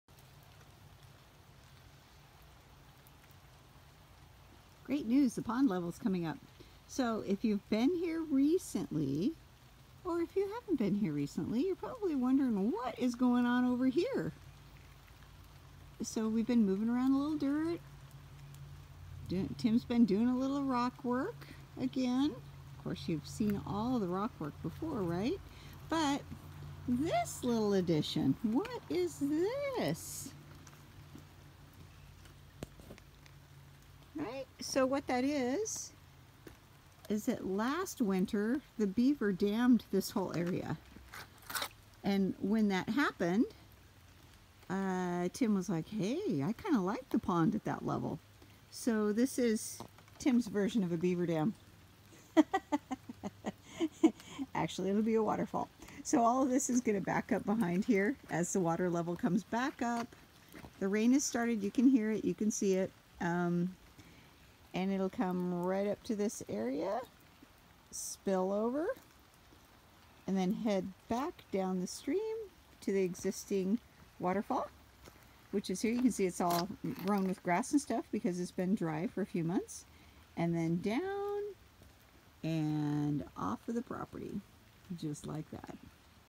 Lower Pond Rock-work Expansion!
All the rock work on our property was built since 2003 by Tim Chovanak. All the individual rocks were selected by him and the mortar mixed by hand in a basic 5 gallon bucket! This video is the most recent addition; a retaining wall waterfall.